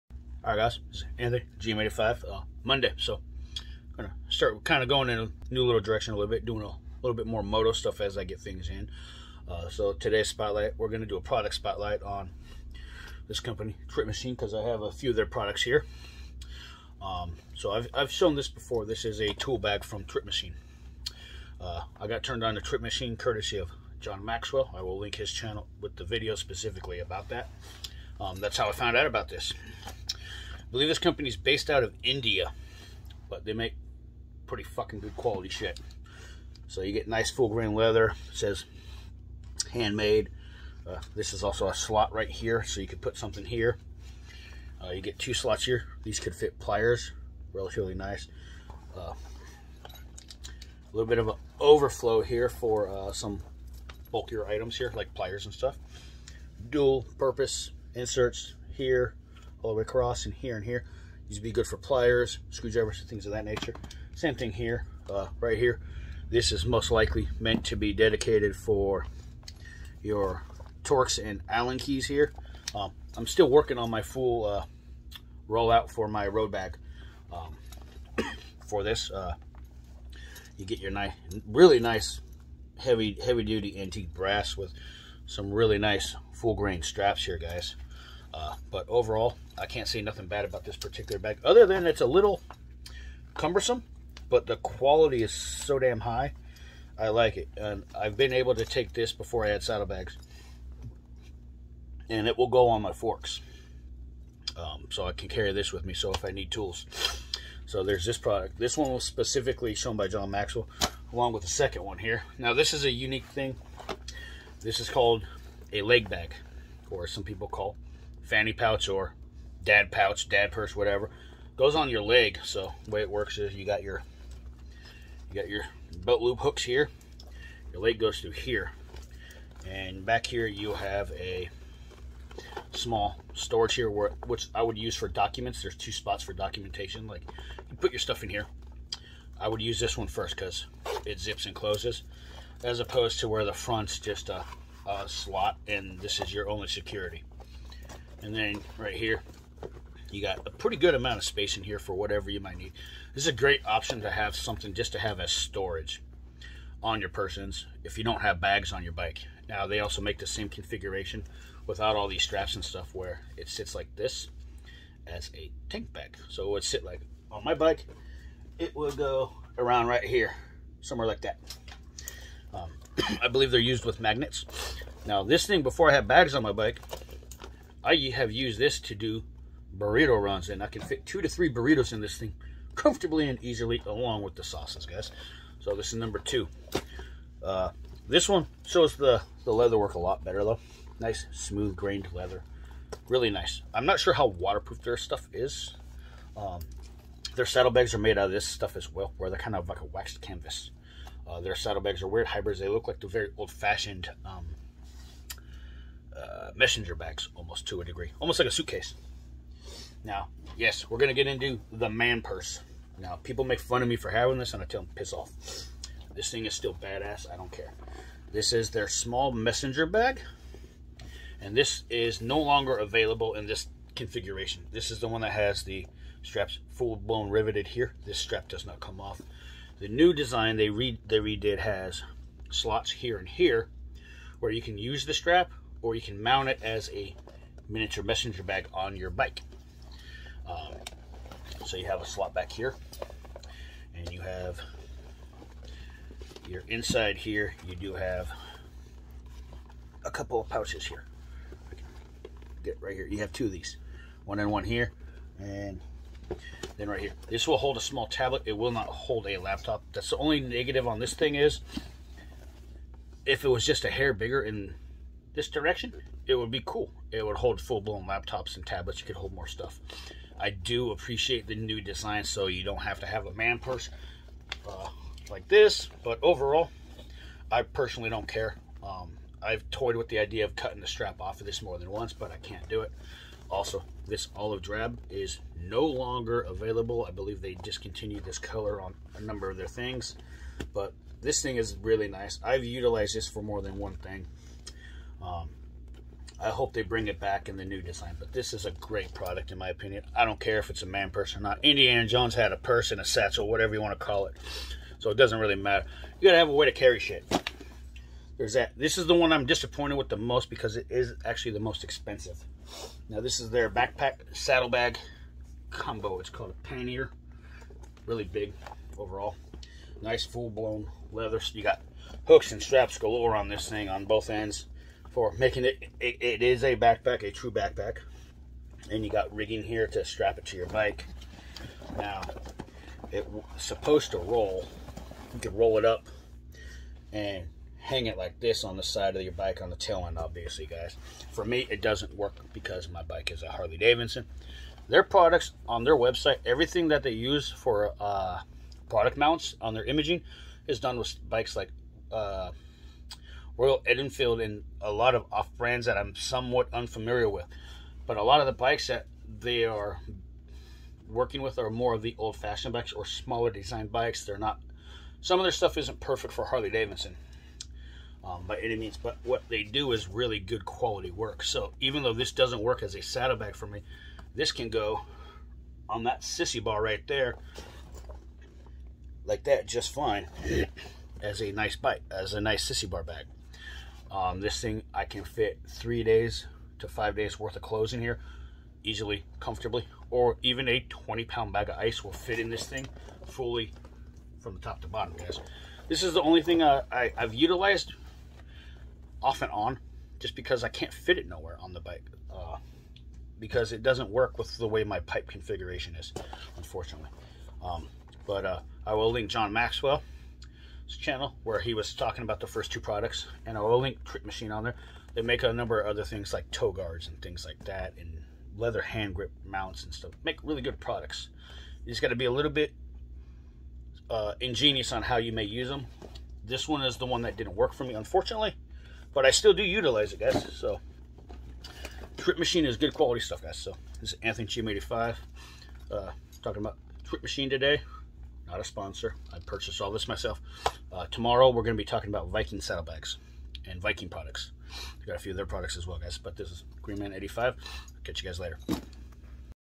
trip machine high quality leathers
small product spotlight on trip machine company
they make saddlebags tank bags message bags and other leather products all high quality

I was turned on to them by John maxwell
he does lots of harley davidson content and owns his own shop, he's not as active as he once was but his knowledge is fantastic

John maxwell trip machine

john maxwell channel